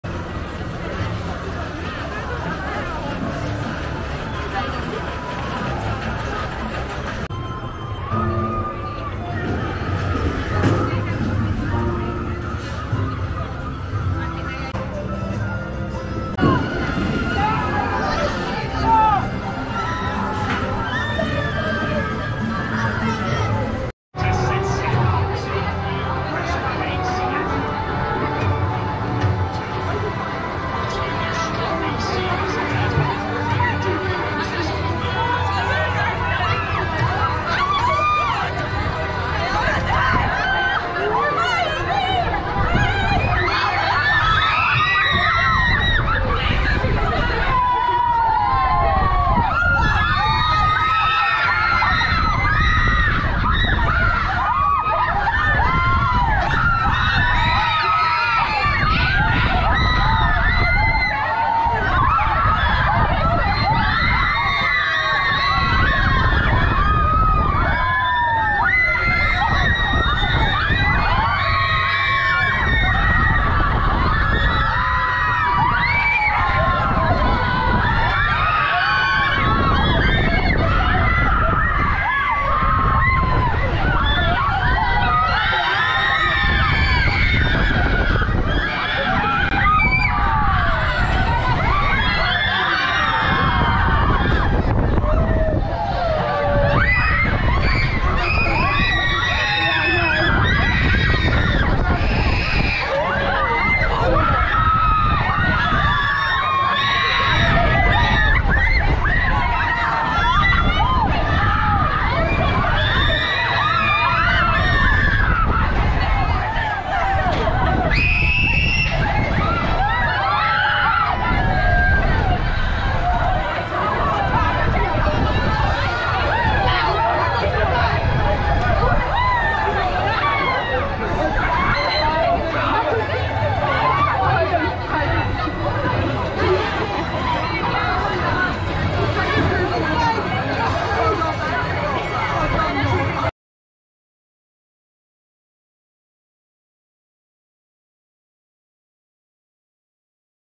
Blue Barnacle On Ride POV - Chessington World of Adventures
New for 2022 is Shipwreck Coast, a brand new nautical land. The land features two brand new rides, Trawler Trouble and Barrel Bail Out as well as two re-imagined classics; Seastorm and Blue Barnacle.

Please note, on ride filming is strictly prohibited at Chessington World of Adventures. We were invited to work with Chessington to film on these rides with our secure camera equipment.